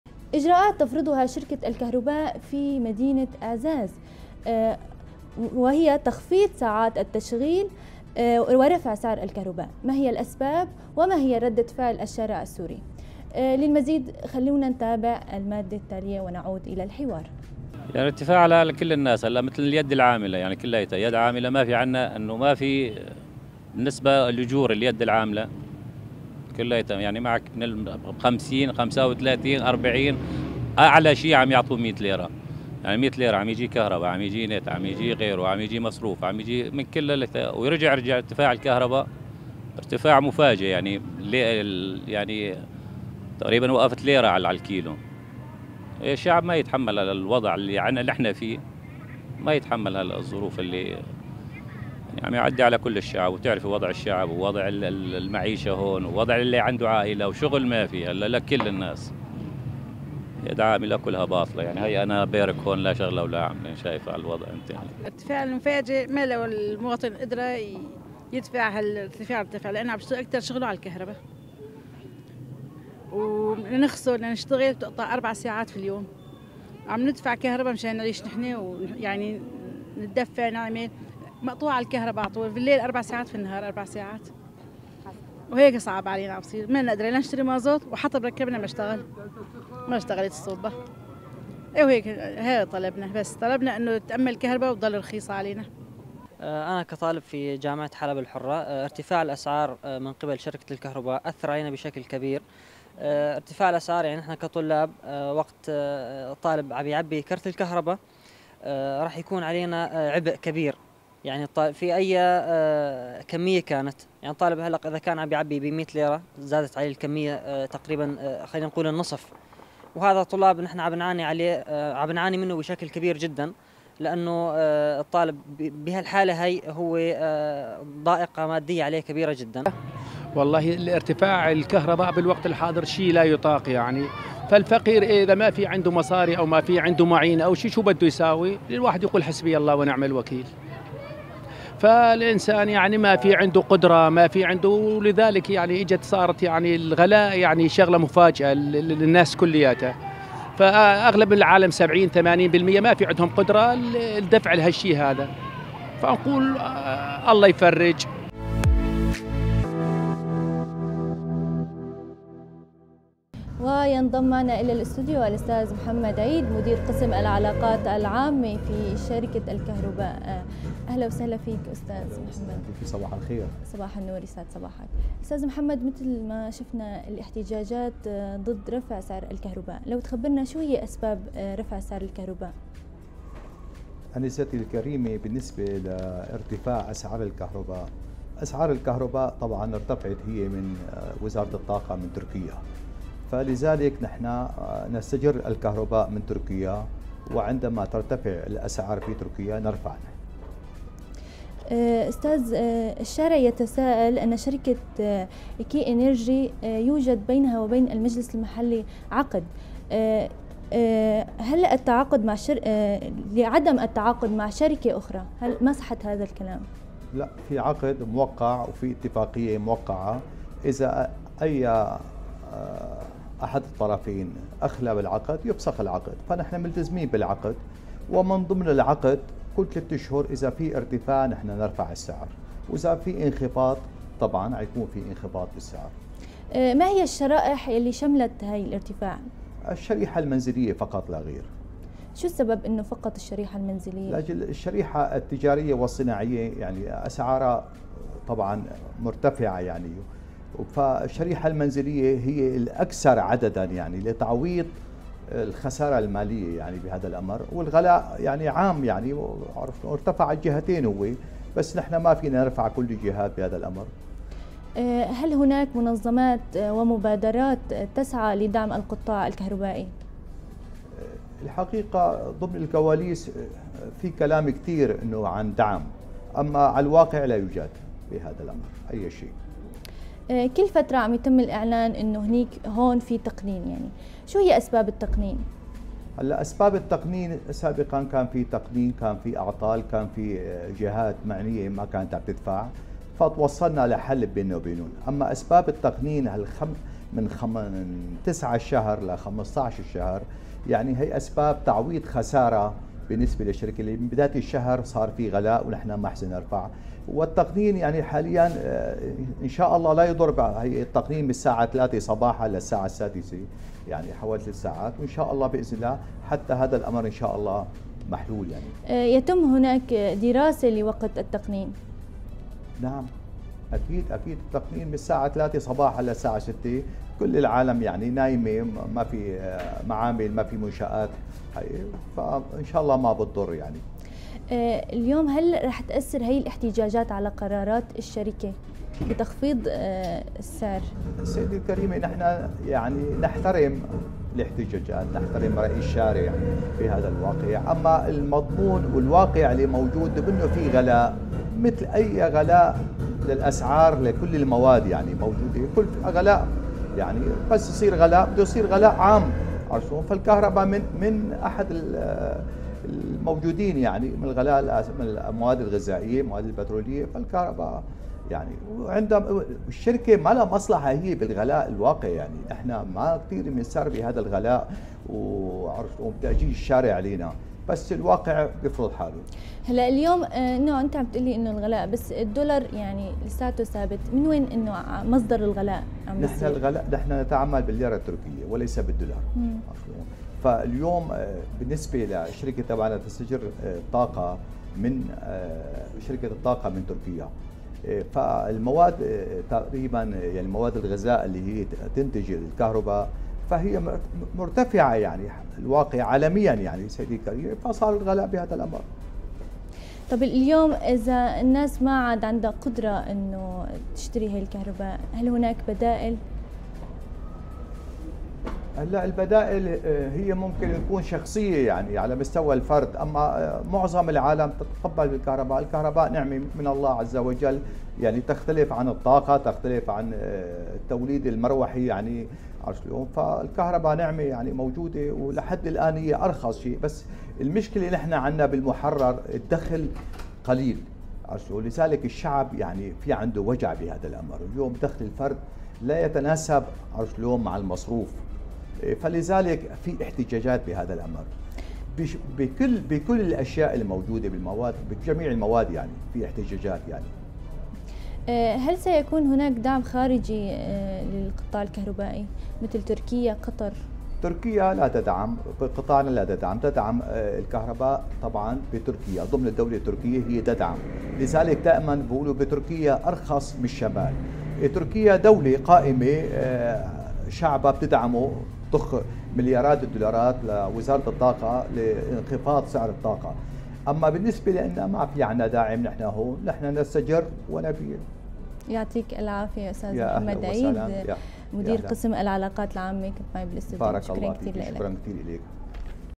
صباح سوري - 12 01 2023 - رفع أسعار الكهرباء في الشمال السوري.
في هذه الفقرة نسلط الضوء على أزمة الكهرباء شمال سوريا وردود فعل المدنيين
منتج محتوى: مي الحمصي
إعداد وتقديم: ابتسام درويش